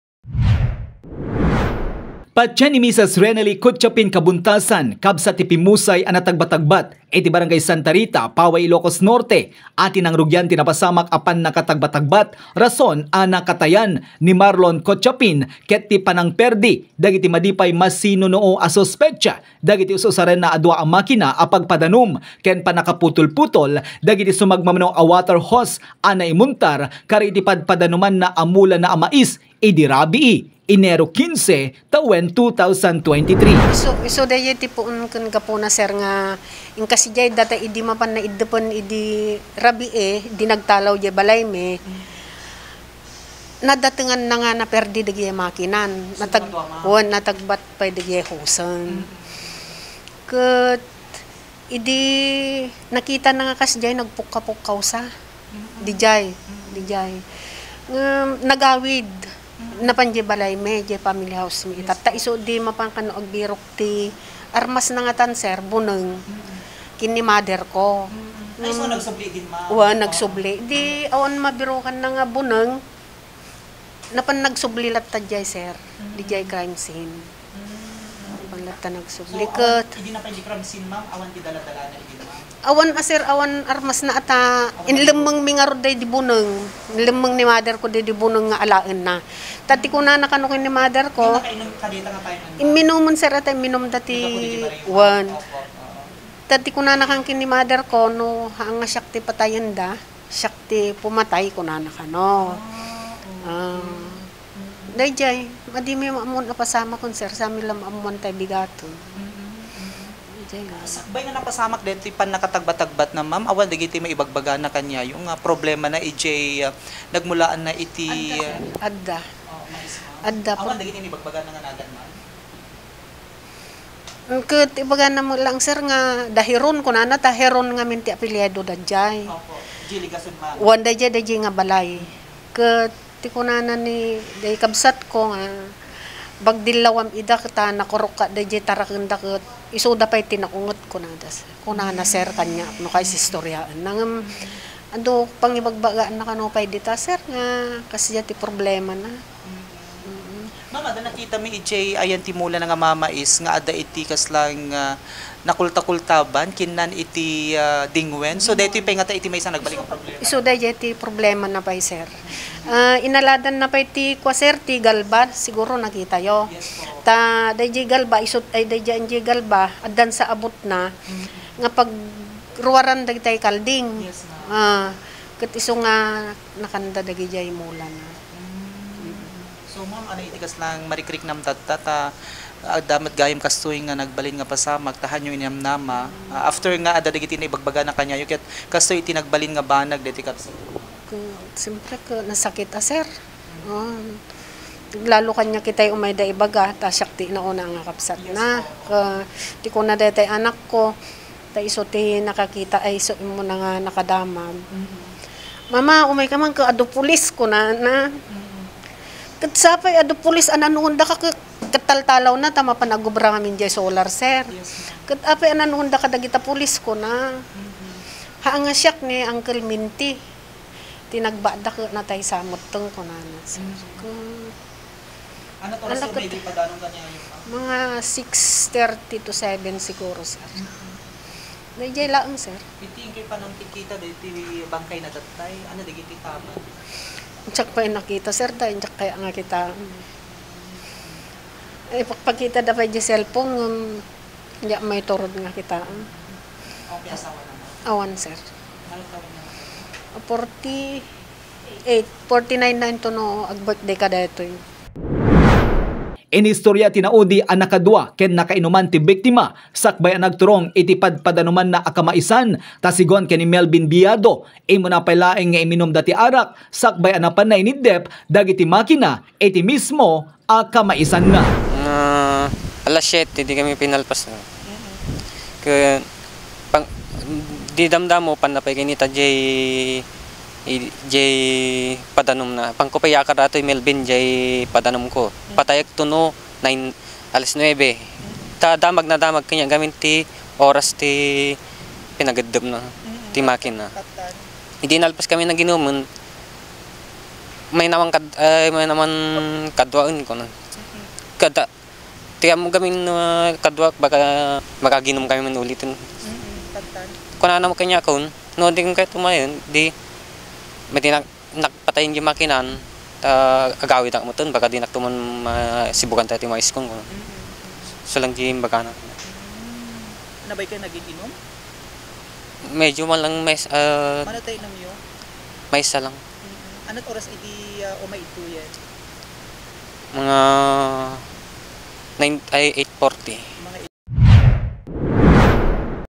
Kabsat ti pimmusay gapo ti pannakatagba-tagbat iti Paoay, inbaga na nga adda dagiti problema na
Inauna a kabsat ti pimmusay gapo ti pannakatagba-tagbat iti Paoay, Ilocos Norte, inbaga na nga adda dagiti naibagbaga na itay kallabes a problema na iti kamaisan na gapo dagiti bulos a dinguen